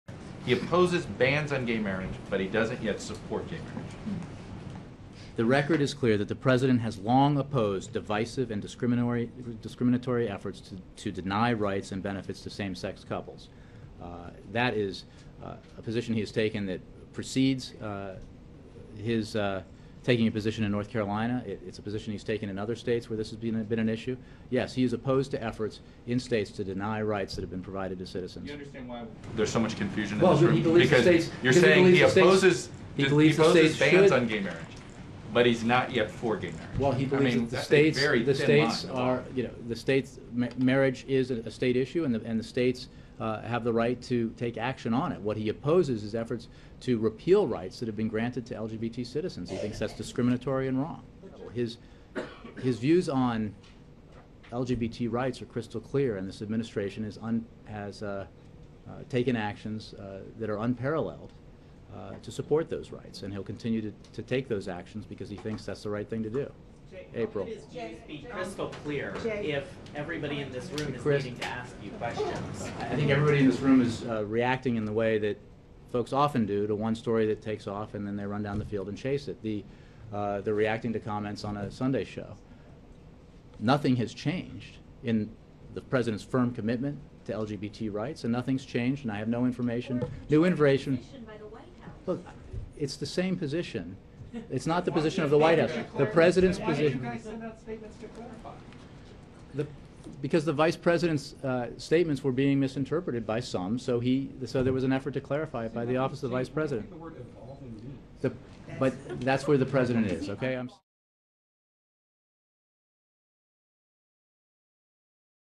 Carney: Obama's views on LGBT rights are clear
White House Press Secretary Jay Carney attempts to clarify President Obama's position on same-sex marriage following a comment by Vice President Biden on a Sunday talk show in support of it.